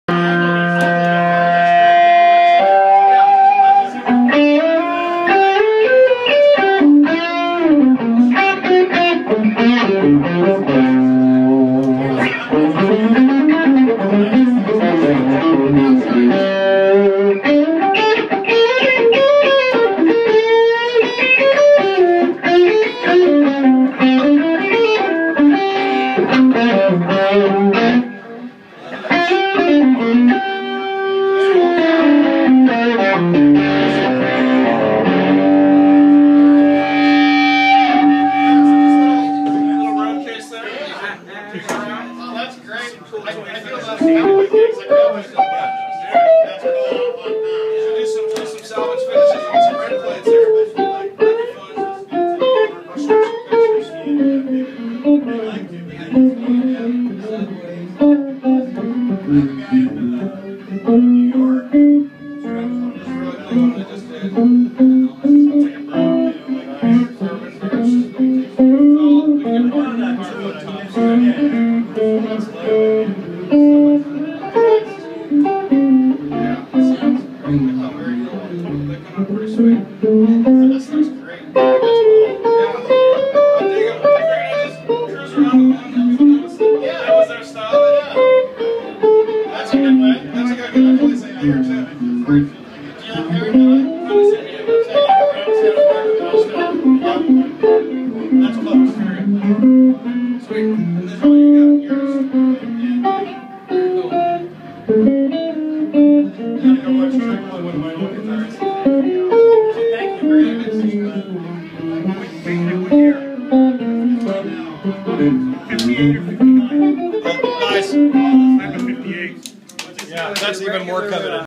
2012 LA AmpShow - RedPlate Amps - with Chuck D'Aloia
Chuck D'Aloia playing a 1959 ES335
Redplate Amplification